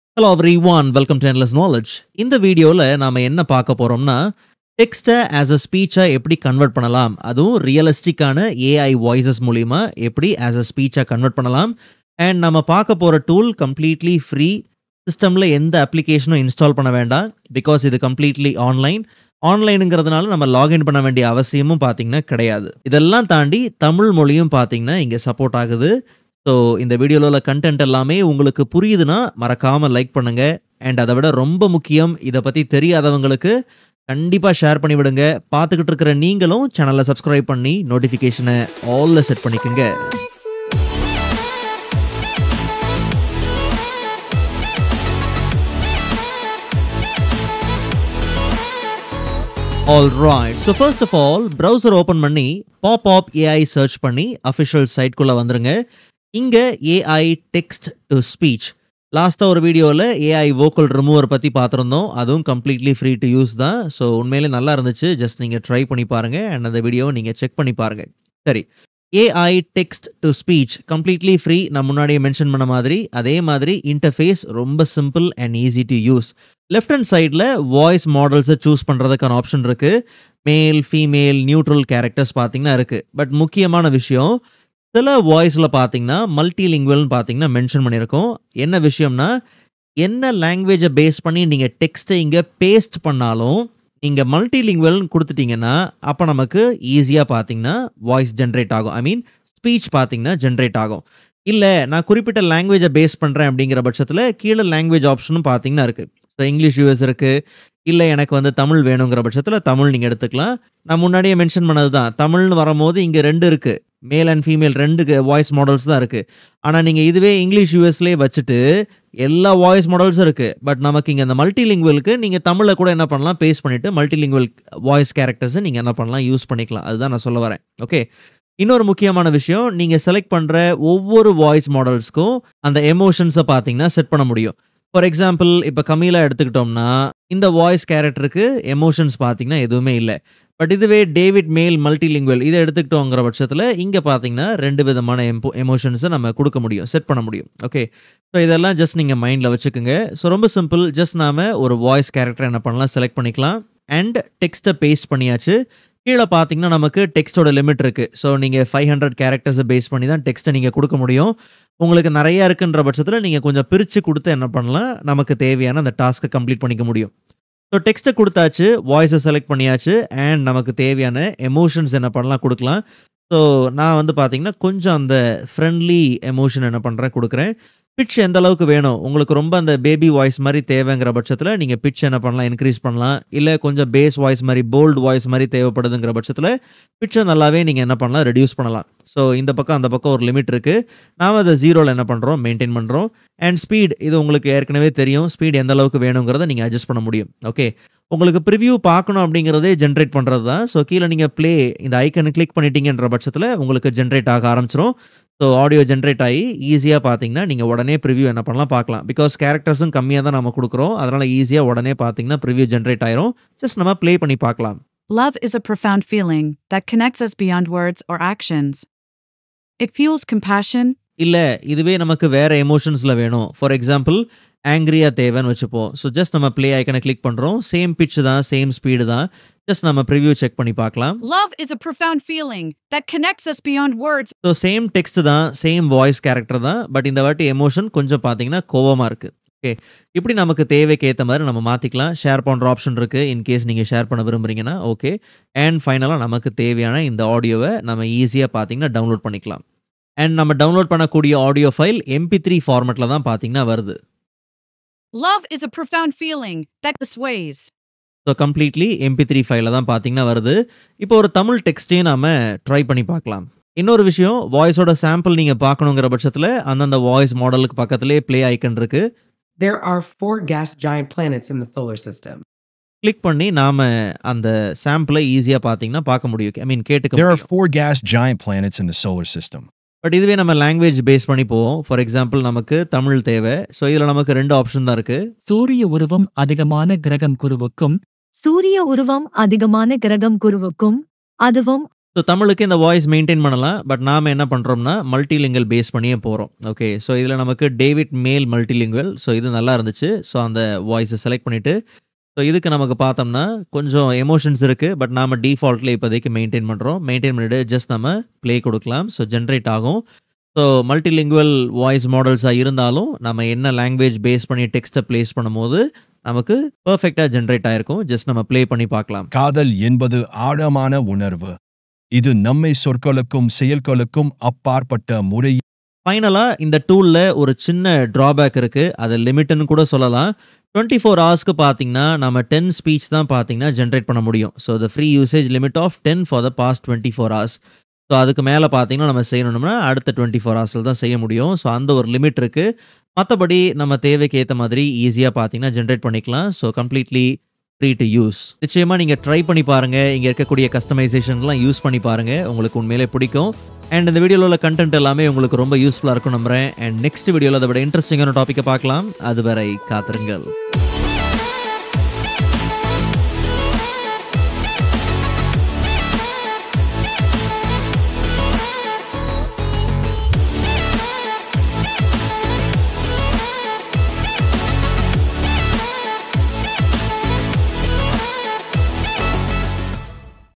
Top Free Text to Speech Converters for Lifelike Audio!
Explore the best Free Text-to-Speech Converters to transform text into lifelike audio! Ideal for videos, podcasts, and presentations, these online TTS tools are easy, free, and offer natural-sounding voices. Perfect for creators and educators! Keywords: Free text-to-speech, TTS converter, AI voice generator.